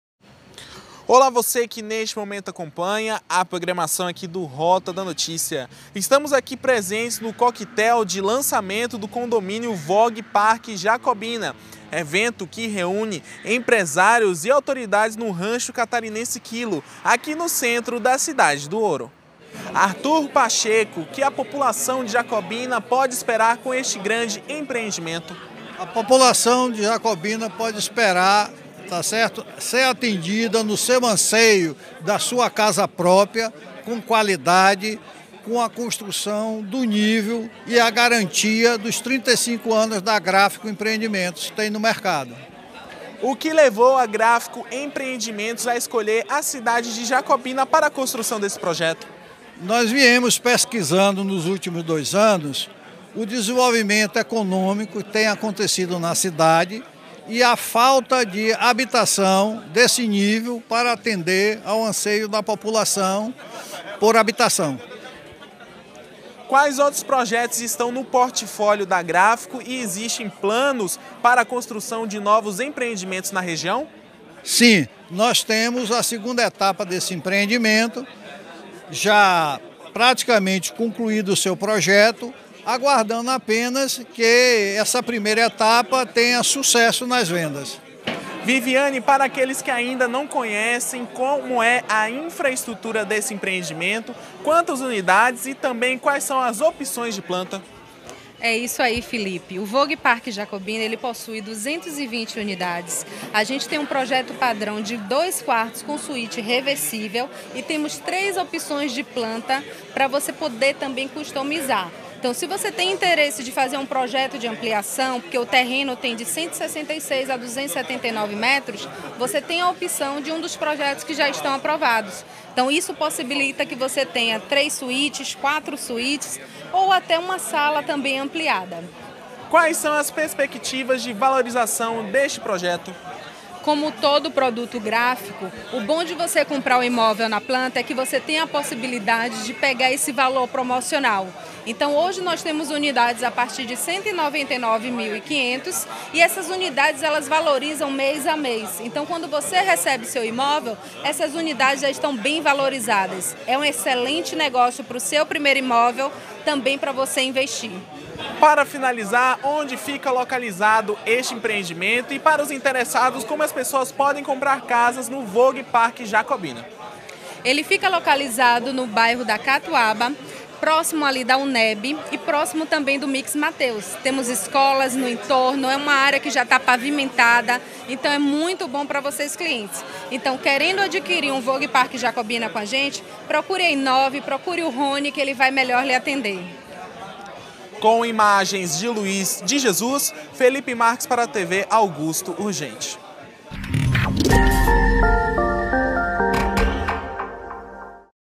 Lançamento do condomínio VOG Park Jacobina, o novo empreendimento imobiliário da cidade do ouro
Nesta terça-feira (16), a Gráfico Empreendimentos realizou o lançamento imobiliário do condomínio VOG Park Jacobina, que rapidamente se tornou um grande sucesso. A empresa superou as expectativas do público, assim como dos compradores em potencial.

O VOG Park Jacobina é um condomínio de casas, que oferece uma infraestrutura completa e moderna, com espaços de lazer e conforto para toda a família. O condomínio tem tudo para ser uma excelente oportunidade de investimento para quem deseja ter a casa própria.

O sucesso do lançamento se deve à uma combinação de fatores, como a excelente localização do condomínio, a qualidade das residências e a infraestrutura completa oferecida pela Gráfico Empreendimentos. Além disso, a empresa é reconhecida por sua credibilidade e compromisso com o cliente, o que gera confiança entre os clientes.

Para quem não pôde comparecer ao evento, ainda é possível conhecer o condomínio e aproveitar as facilidades de pagamento oferecidas pela Gráfico. Com a crescente valorização imobiliária na região, investir no VOG Park Jacobina é uma excelente oportunidade de adquirir a casa própria e garantir um futuro tranquilo para toda a sua família.

Para mais informações você pode entrar em contato com o número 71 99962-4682, no WhatsApp.